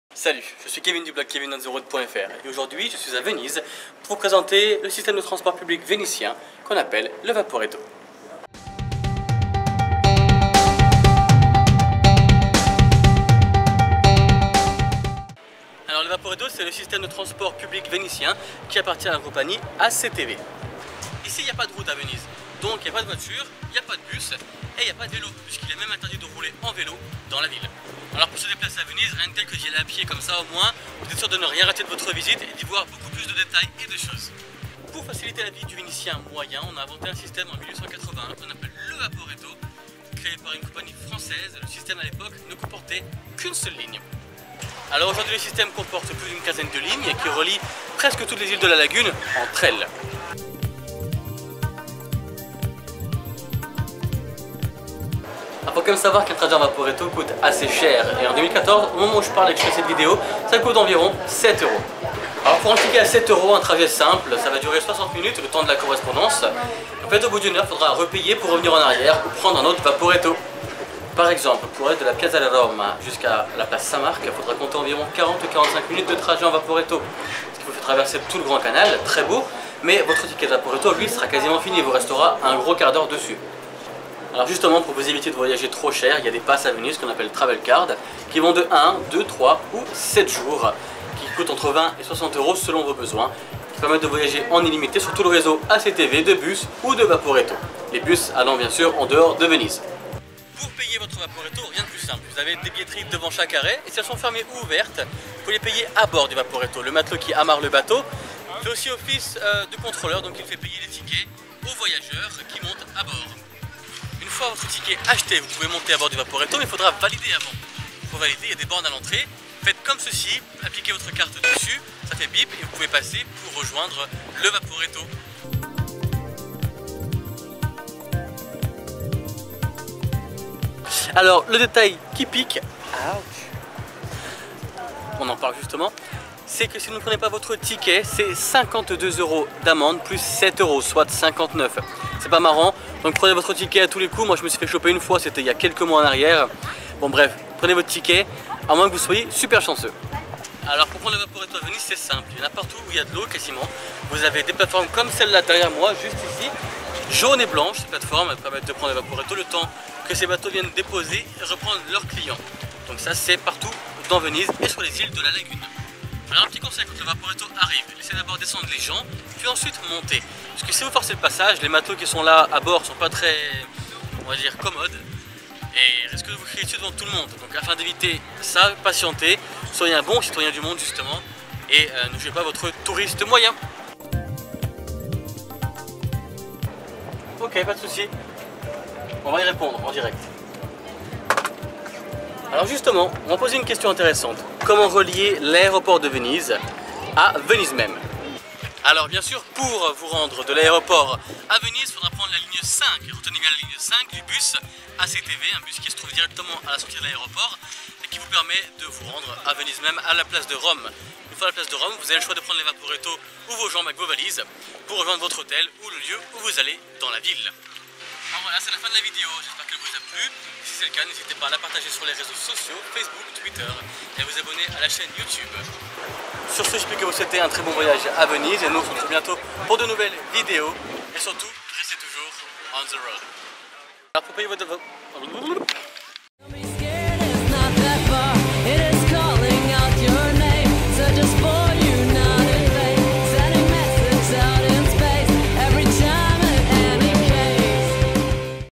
Visiter Venise - Comment utiliser le Vaporetto ?
Dans cette vidéo je vous explique comment utiliser le Vaporetto à Venise, le transport public de la ville.
Prix, stations, détails et conseils pour ne pas se perdre dans la cité des Doges.